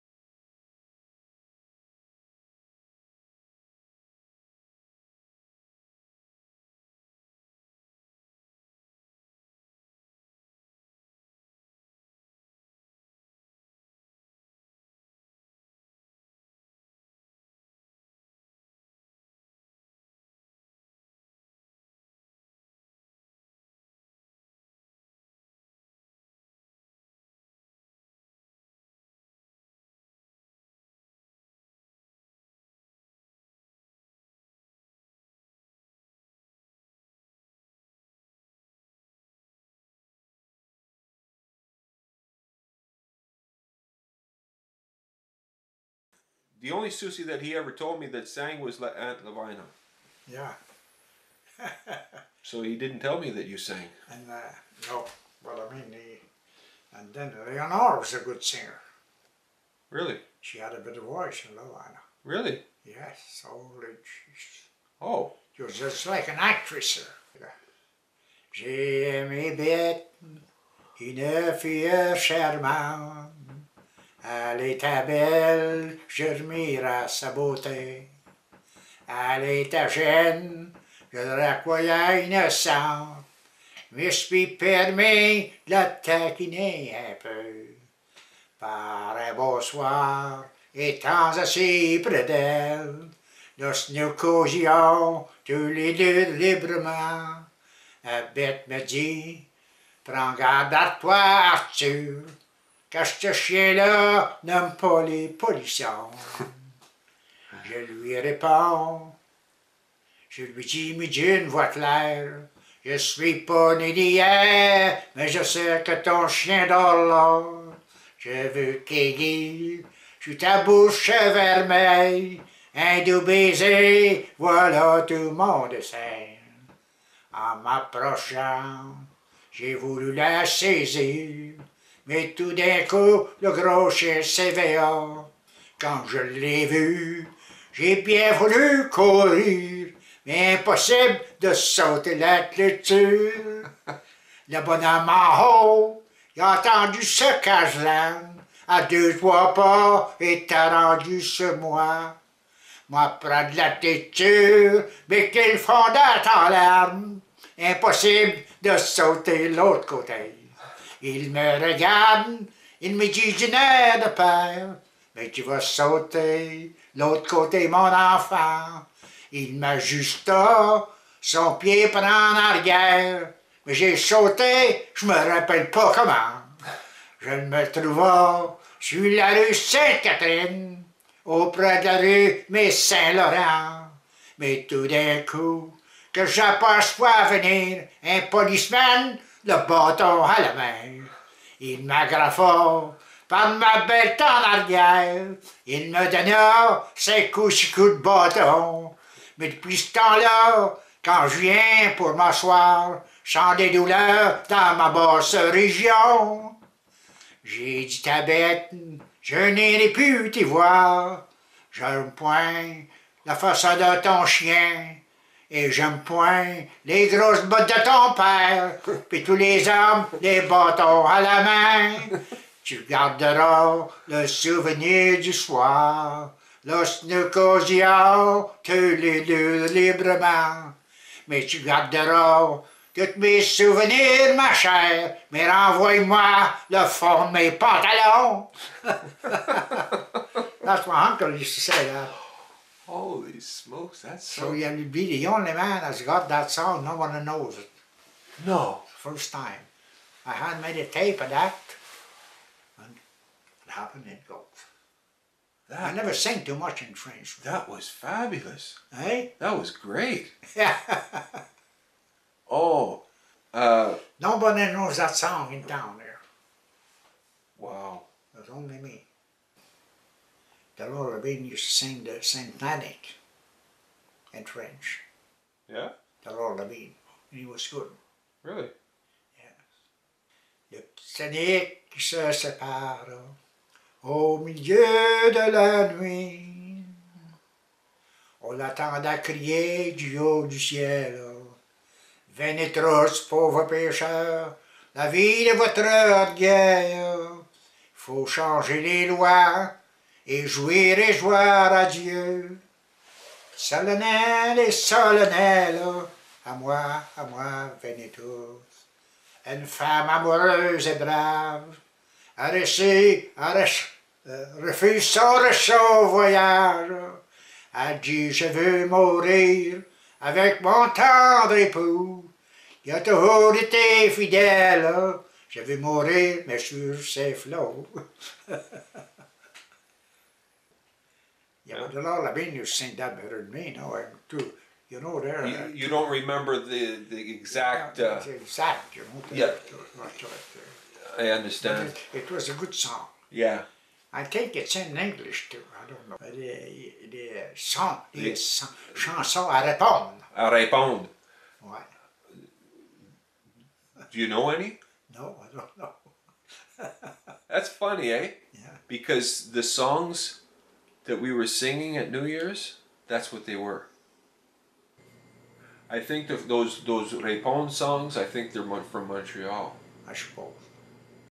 Uncle Achille singing a French song at 93 years of age
2013 - Fort Coulonge